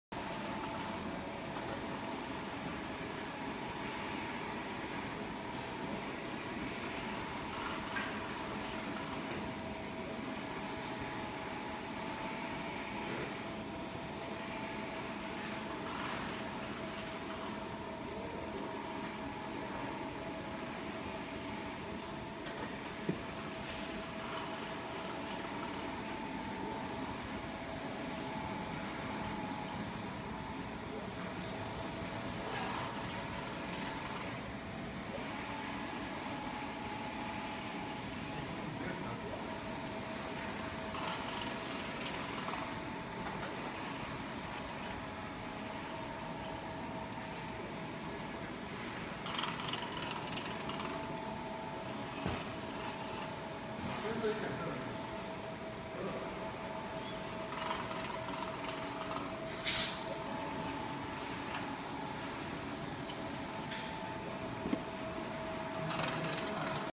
3.2m eco solvent printer dx5 head printing
Shanghai Yorkdeal Digital Technology Co.,Ltd
skype:alice896411.
This is 3.2m dx5 eco solvent printer printing process,this material is wallpaper.You can also print on pp paper,photo paper,self adhesive vinyl,one way vision,backlit Pet film and so on.